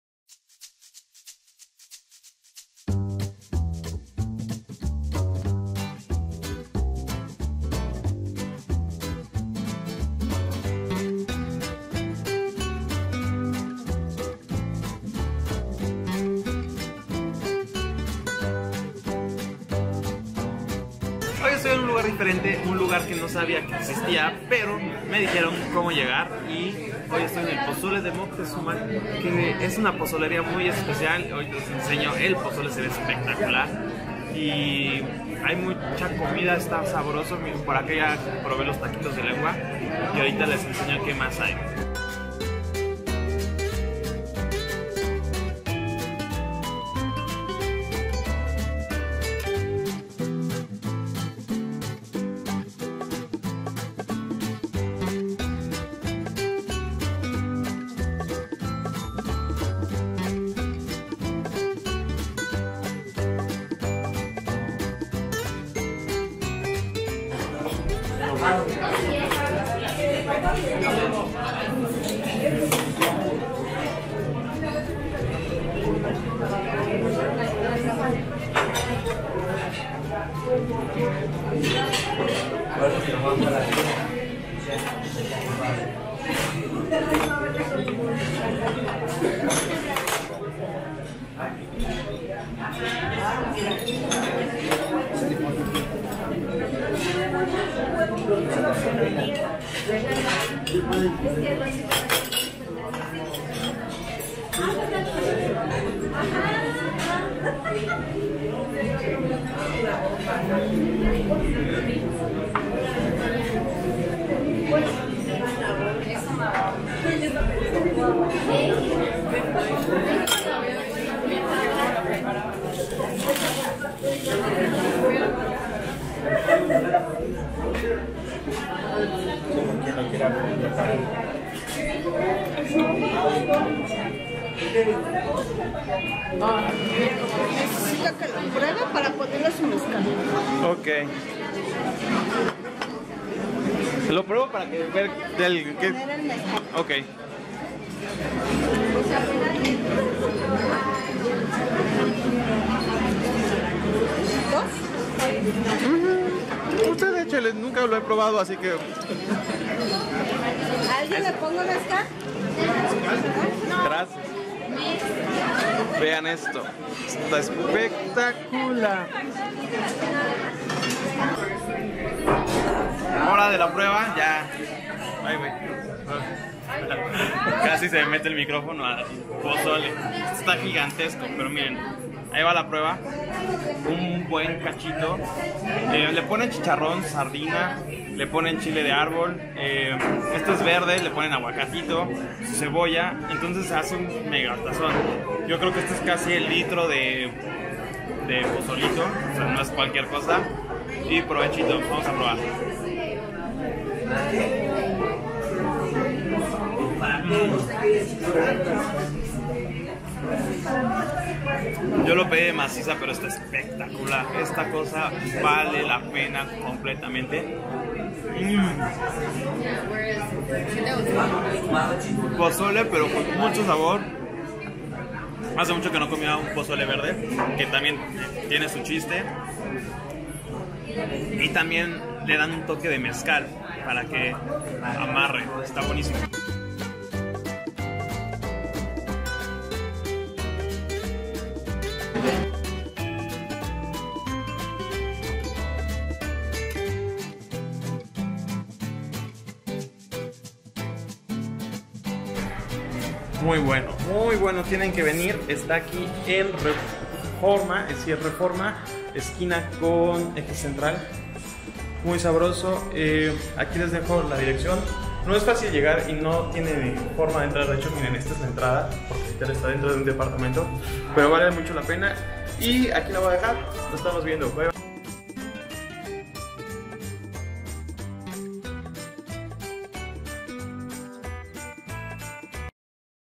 El Pozole de Moctezuma
Los invito a conocer uno de los mejores pozoles que se pueden encontrar en la CDMX.